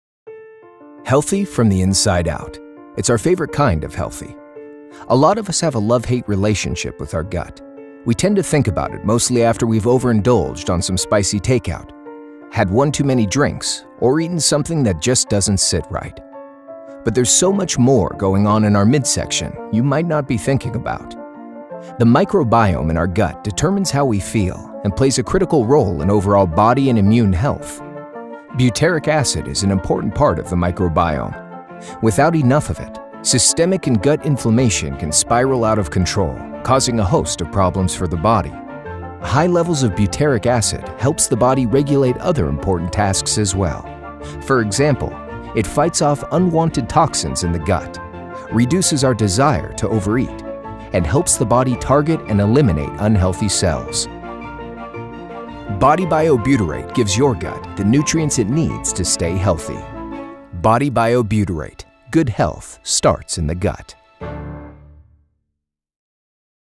Butyrate and Gut Health
Butyrate is a keto-friendly short chain fatty acid that encourages the formation of friendly bacteria in the gut. Modern living and our daily choices disrupt our microbiome and compromise our health beyond simple digestive issues. Butyrate is fuel for a healthy gut.  Feed your microbiome the right stuff.
- Improves gut function*
- Supports a healthy inflammation response*
- May help burn fat*
- Cleans the liver & gallbladder*
- Helps to controls ammonia in the gut*
- Suppresses the desire to overeat by increases leptin production*
- Promotes healthy microbiome*